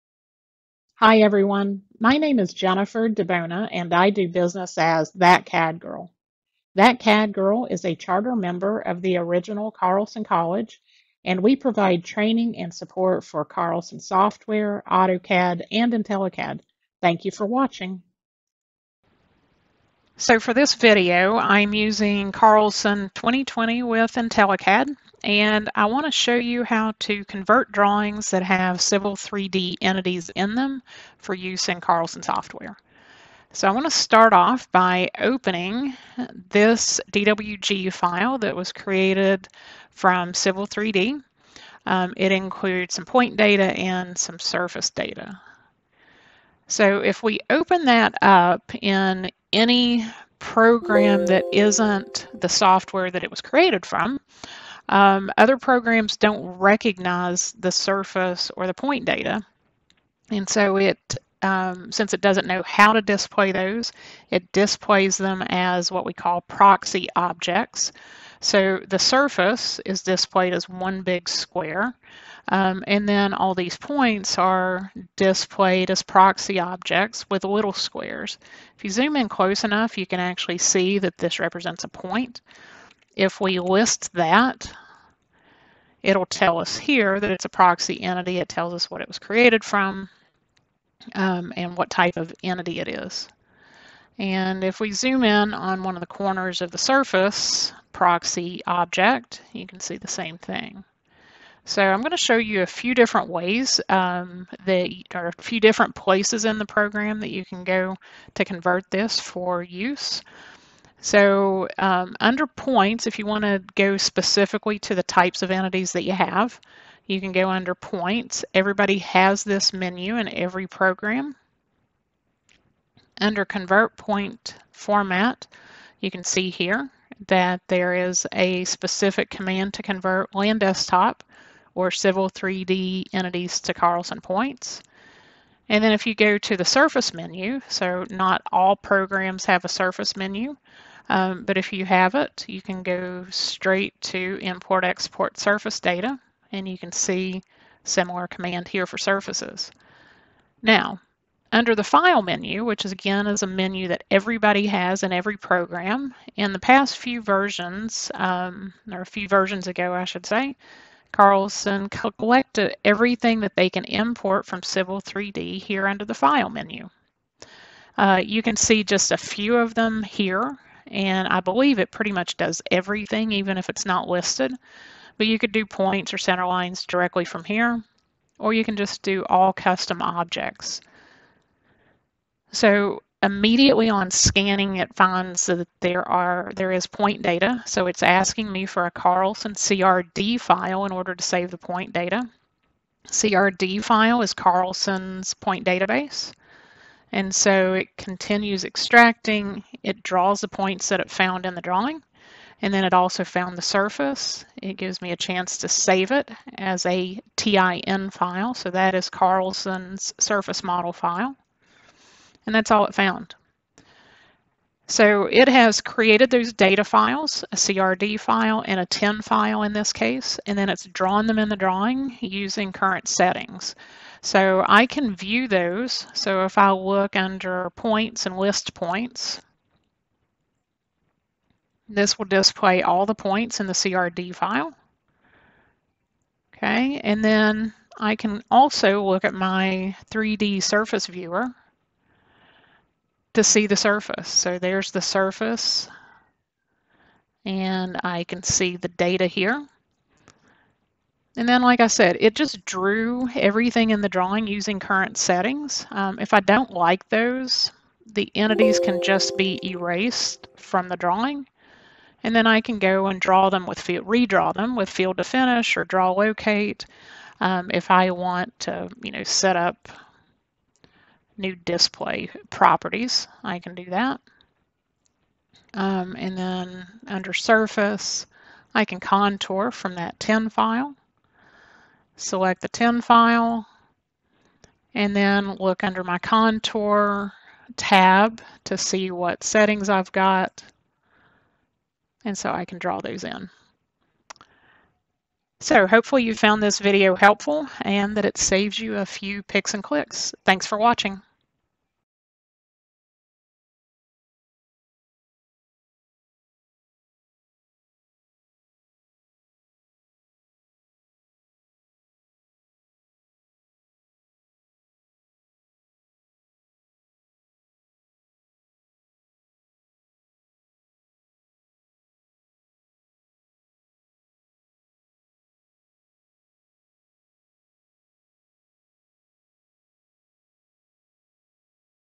Converting Civil 3D Objects into Carlson Data Files
This video was created using Carlson 2020 with IntelliCAD.

This video will show you how to use the tools in Carlson's desktop software to import (or export) data files such as points, surfaces, profiles, sections and more between Carlson and Civil 3d. These conversion routines are available in all modules of Carlson Software.